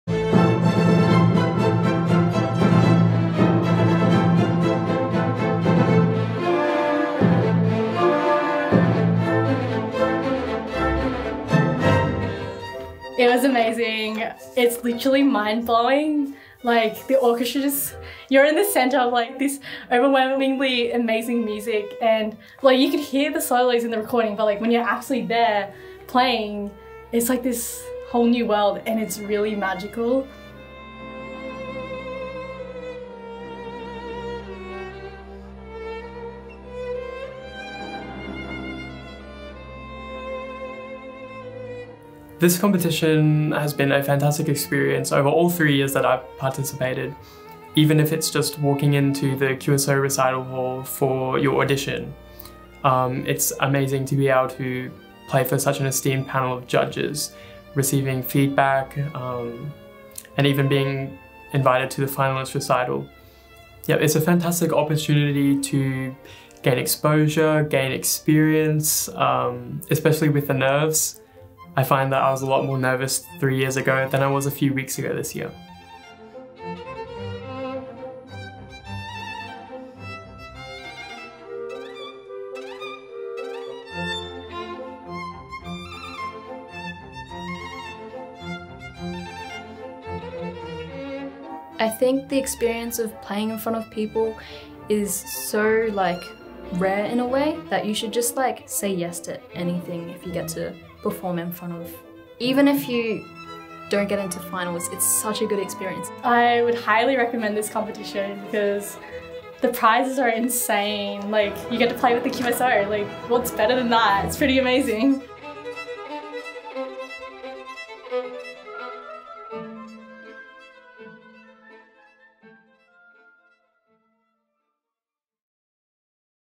2021 Young Instrumentalist Prize
The Young Instrumentalist Prize provides performance experiences and cash prizes to exceptional Queensland school instrumental students. It has launched the performing dreams of many young musicians.

The Young Instrumentalist Prize is proudly supported by Brisbane Airport Corporation, CP Morris, and Australian Decorative and Fine Arts Societies (ADFAS), Brisbane.